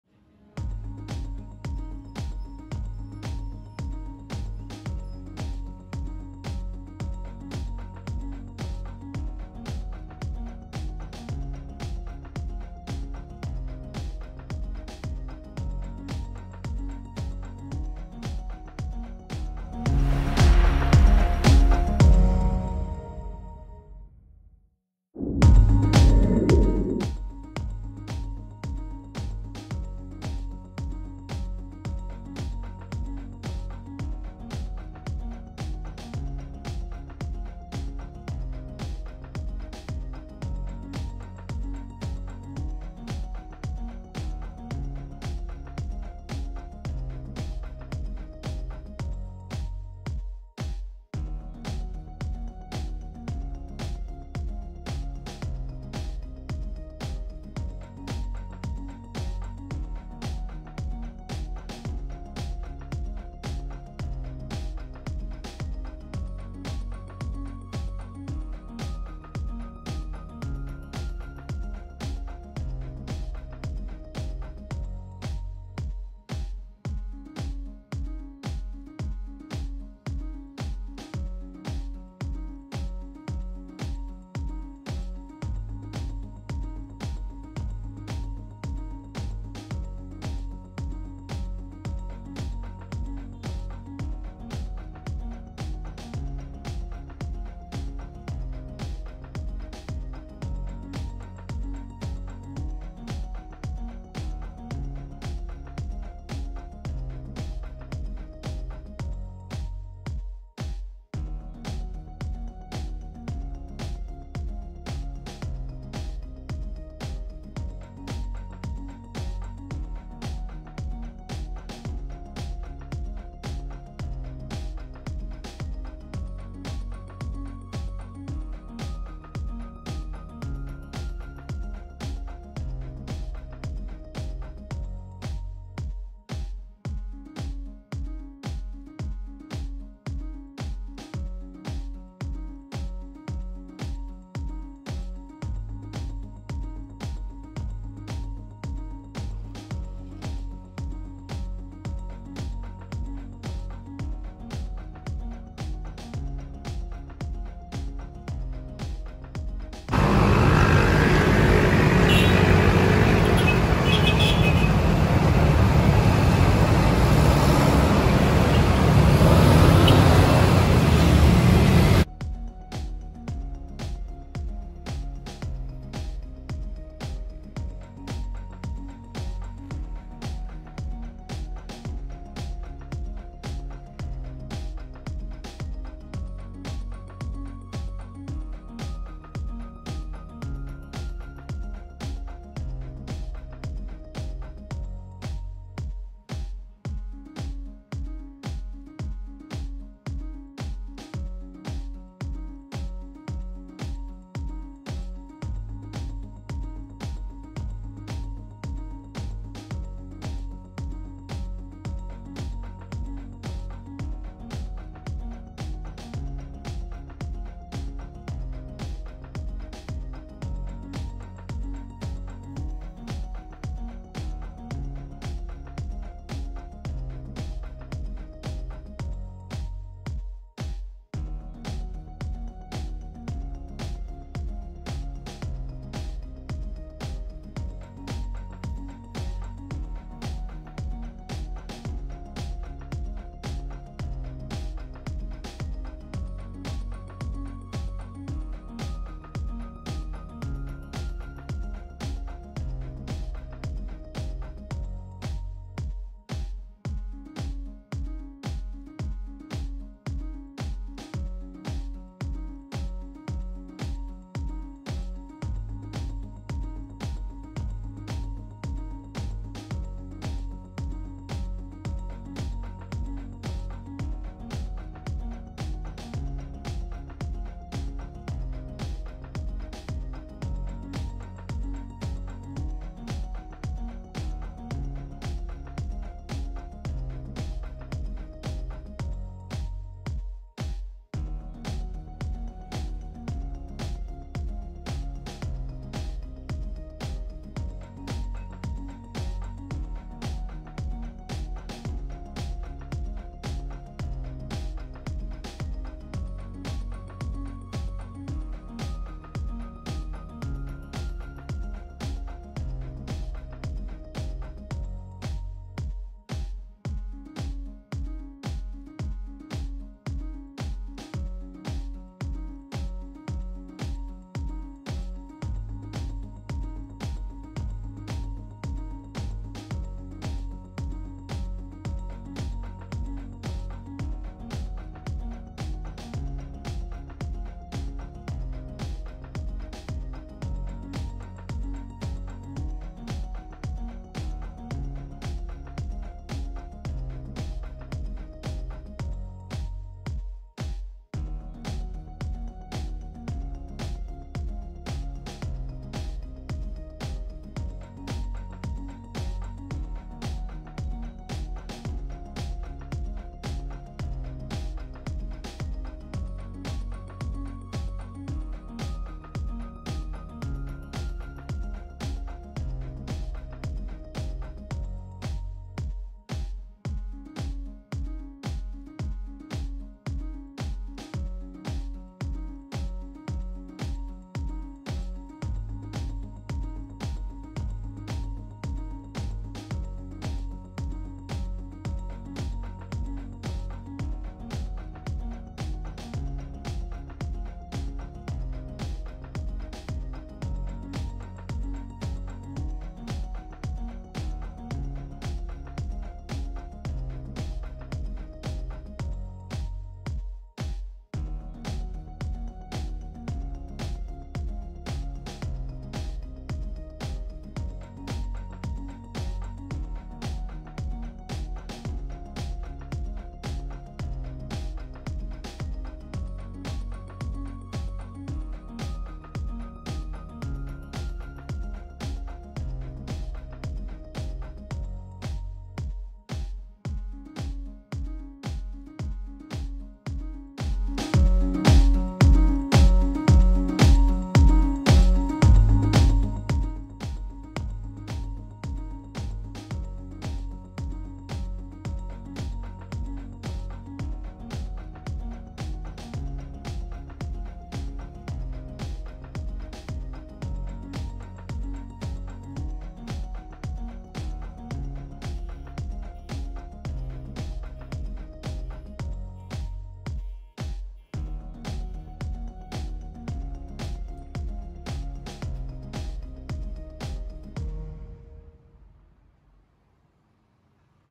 Parul Sampernandu | The Magnificent Christmas Lanterns of San Fernando, Pampanga!
A look at the well-known Parul Sampernandu along the Jose Abad Santos Avenue in San Fernando City, Pampanga.